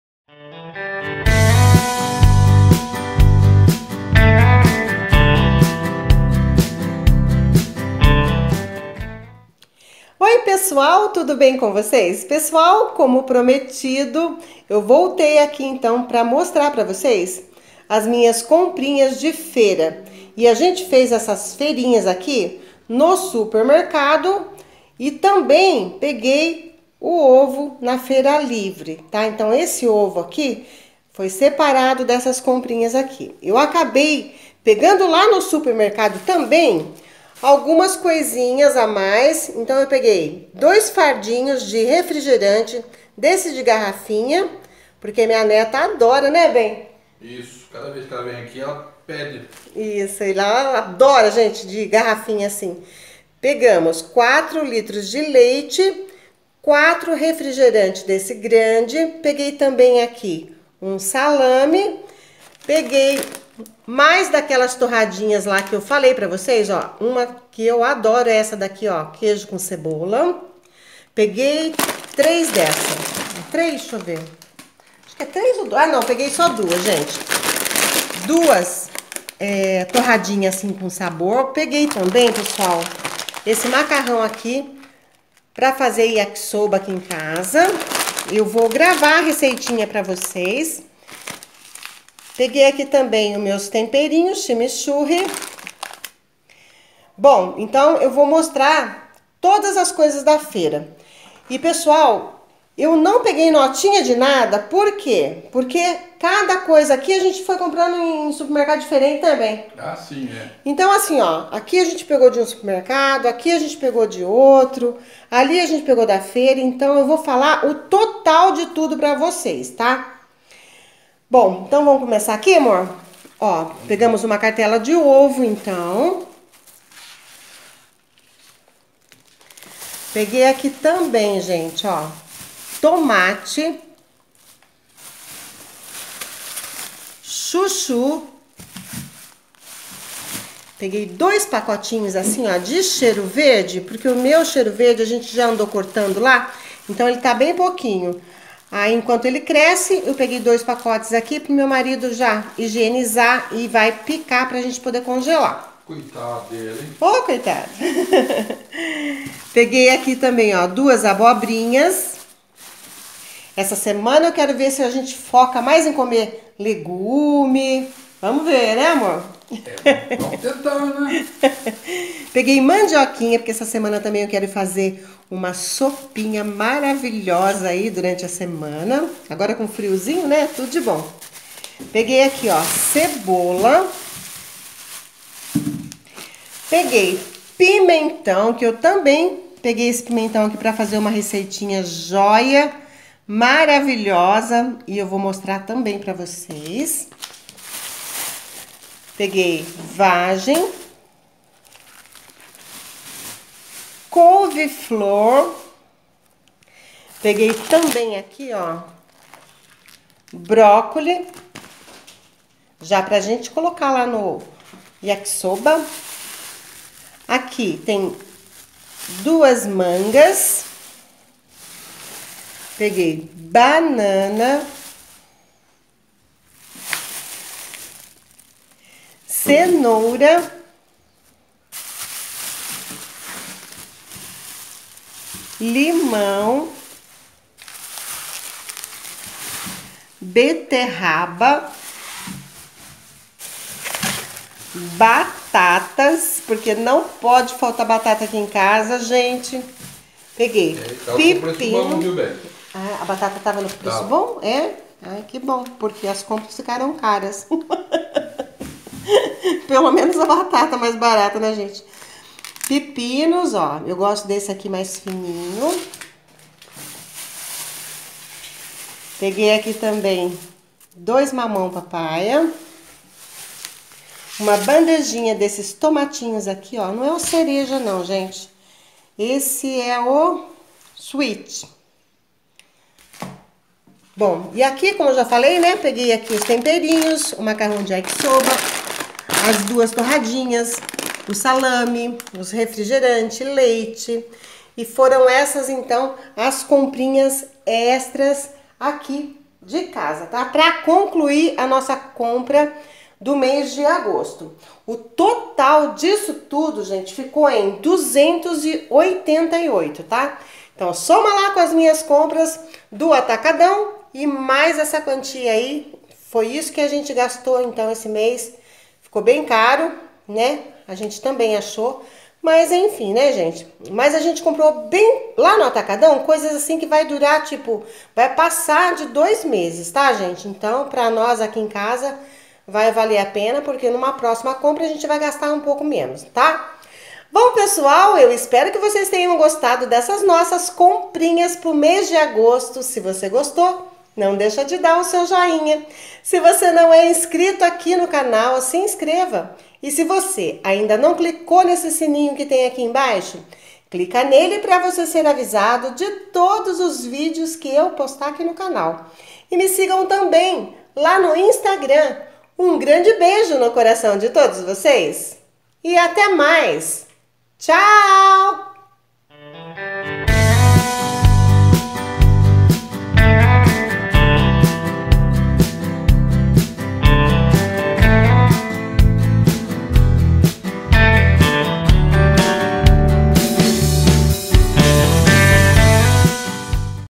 Compras de feira + bebidinhas
Oi pessoal,neste valor das compras de feira também está incluso as carnes que comprei e me esqueci de mostrar no video,elas foram :
600 grs de peito de frango
600 grs de carne de porco
600 grs de contra filé

❤️MEUS CONTATOS:
Quer me enviar cartinhas ou mimos,fale comigo
pelo email !